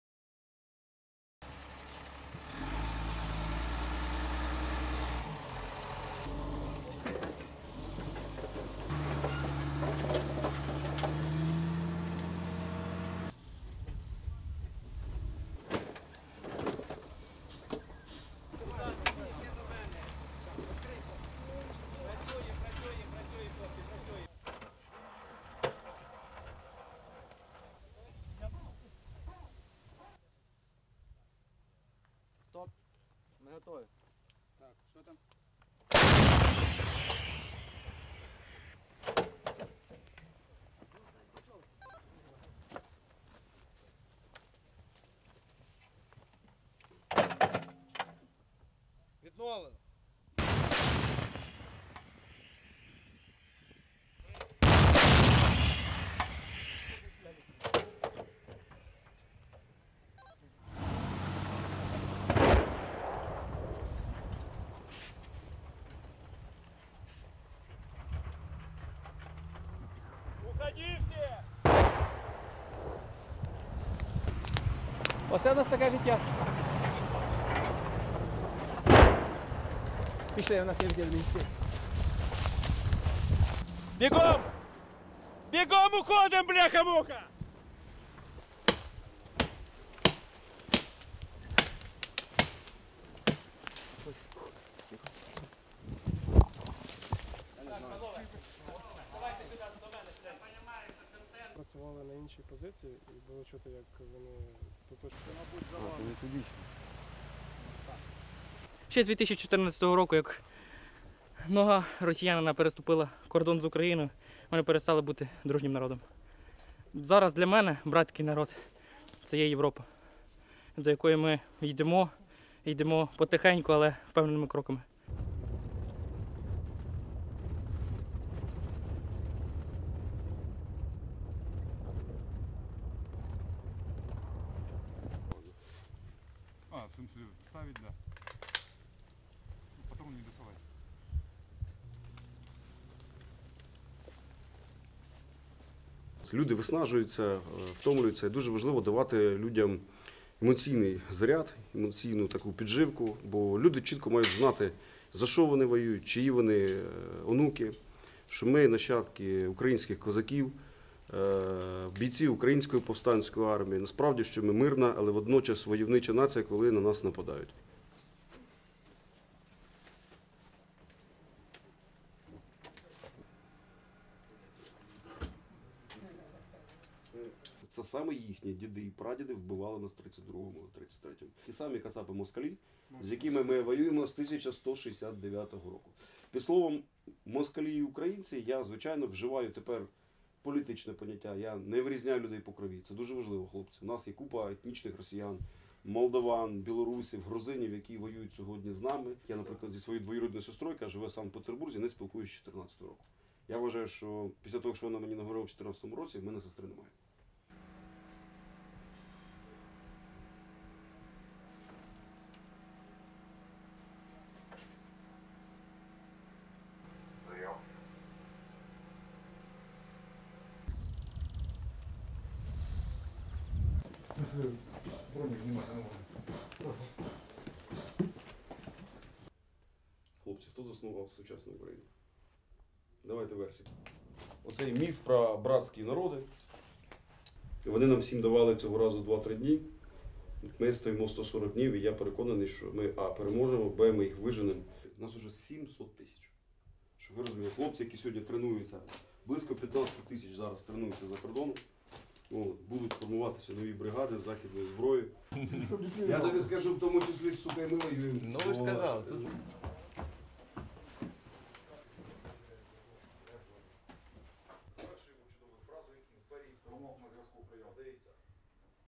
Ukrainian forces use US howitzers on battlefield
RESTRICTION SUMMARY:
++CLIENTS PLEASE USE THIS VERSION OF EDIT WHICH ADDS SHOT OF BATTLEFIELD IN SHOT 10++
ASSOCIATED PRESS
Kharkiv region - 14 July 2022
1. Various of Ukrainian servicemen arriving in field with M777 howitzers
2. Various of servicemen setting up howitzers, firing artillery
3. Vehicles with howitzers driving away, camera pans UPSOUND: shooting
4. Ukrainian serviceman looking into the battlefield amid Russian shooting UPSOUND (Ukrainian): "That's how our life looks like."
5. Servicemen and journalists run away from the battlefield
6. Serviceman cuts branches.
7. Servicemen hide the vehicle with branches
8. Various of servicemen inside the forest
9. SOUNDBITE (Ukrainian) Nom de guerre "Enot", Ukrainian serviceman:
"Since 2014, when a Russian foot crossed the border with Ukraine, we have ceased to be a friendly people. Now, for me, a fraternal nation is Europe. To which we are going, we are going slowly, but with a certain step."
ASSOCIATED PRESS
Kharkiv region - 12 July 2022
10. Aerial shot of battlefield ++MUTE++
11. Drive-by of damaged Ukrainian village on the outskirts of Kharkiv
12. Various of Ukrainian servicemen of Khartiia battalion walking through the forest
13. SOUNDBITE (Ukrainian) Roman Semenukha, Ukrainian serviceman, historian:
"People (servicemen) get exhausted, tired, and it is very important to give people an emotional charge, emotional support. People should know what they are fighting for, whose grandchildren they are. That we are descendants of Ukrainian Cossacks, fighters of the Ukrainian rebel army. In fact, we are a peaceful nation, but at the same time a warlike nation when we are attacked."
14. Ukrainian servicemen in room at their base
15. Close of Ukrainian serviceman
16. Ukrainian servicemen listening to historical lecture UPSOUND (Ukrainian): "It was their grandfathers and great-grandfathers who killed us in 33-34."
17. Close of Roman Semenukha, UPSOUND (Ukrainian): "The same Katsap (Russians) and Muscovites with whom we have been fighting since 1169. Under the words Muscovites and Ukrainians, I now use a political concept. I don't distinguish people by blood. This is very important guys. We have a lot of ethnic Russians, Moldovans, Belarusians, Georgians who are fighting with us today."
18. Various of Ukrainian servicemen listening UPSOUND (Ukrainian): "For example, I have not communicated with my cousin in St. Petersburg since 2014. I believe that after what she told me in 2014, I don't have a sister."
19. Various of servicemen
20. Various of damaged building
21. Ukrainian servicemen going down to the basement
22. Ukrainian servicemen of Khartiia battalion listening to historical lecture inside the basement
23. Close of Roman Semenukha
24. Various of servicemen listening
25. Ukrainian servicemen up the stairs
26. Flag of Khartiia battalion
STORYLINE:
Ukrainian servicemen on Thursday used American M777 howitzers on the battlefield in the northeast of the war torn country.
The United States last month sent Ukraine another batch of the M777 155-mm towed howitzers.
The artillery systems are part of the US' military aid package to combat Russia's full-scale invasion which began in February this year.
Depending on the type of projectile used, the M777 howitzer, which has a firing rate of up to five rounds per minute, has a range from 24 to 30 kilometers (15 to 18 miles).
More than 30 countries have given huge amounts of military equipment to Ukraine.
"Now, for me, a fraternal nation is Europe. To which we are going, we are going slowly, but with a certain step," he added.